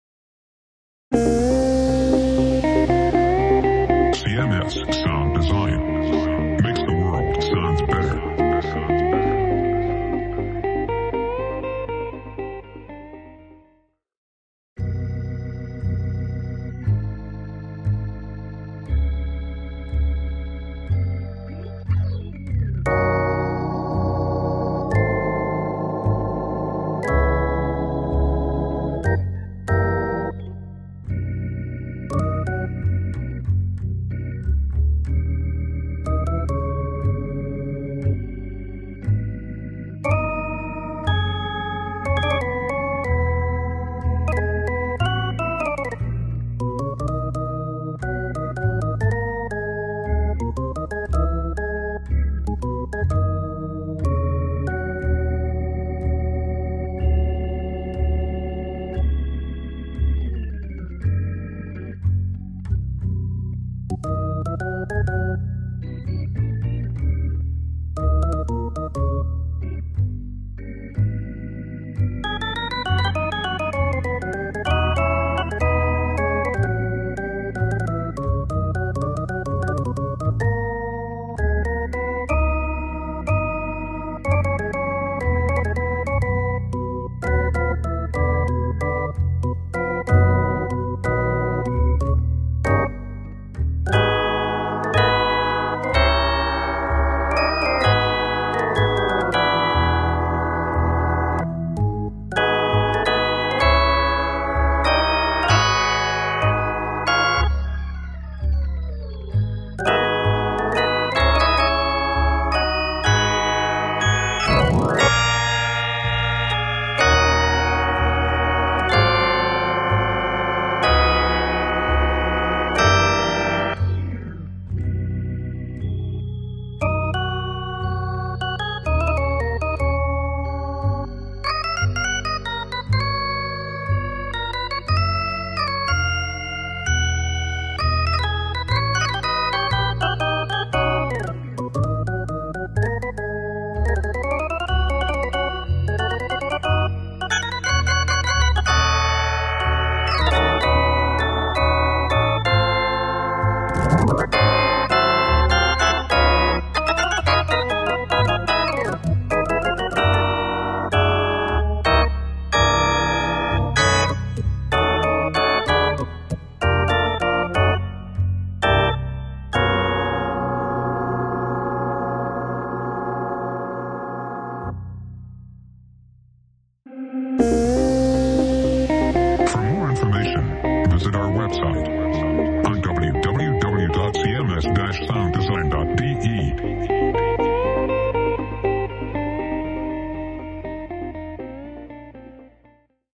CMS-Sounddesign - CMS Wersi Helios Freeware - Audio Demo
Sounds: CMS Wersi Helios Freeware ... aus dem Sortiment von CMS-Sounddesign Freeware Einzelsounds für den Yamaha Expansion Manager (YEM).

Audio Aufnahme: UAD Apollo 8p DSP Recording System
Instrument: Yamaha Tyros 5

CMS Wersi Helios Freeware